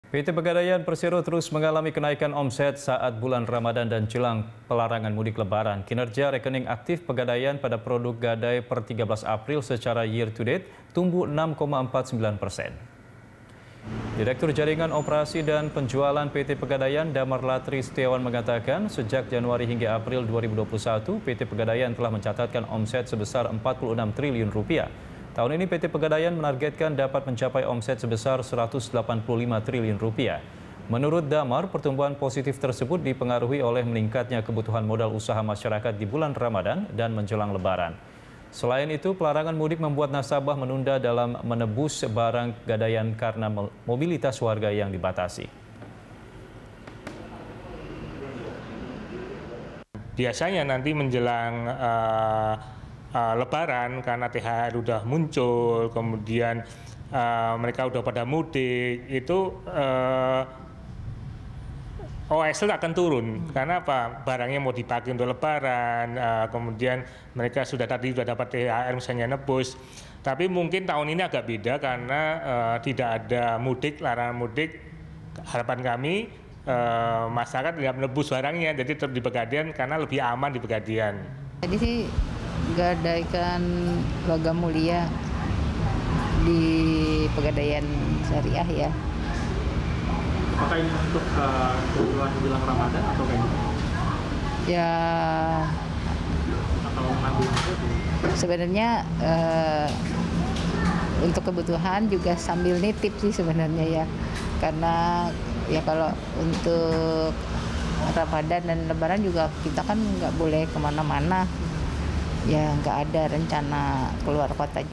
Pegadaian Catatkan Kenaikan Omzet Selama Ramadan dan Jelang Lebaran
PT Pegadaian Persero terus mengalami kenaikan omzet saat Ramadan dan jelang pelarangan mudik. Kinerja rekening aktif pegadaian pada produk gadai per 13 April secara year to date tumbuh 6,49 persen.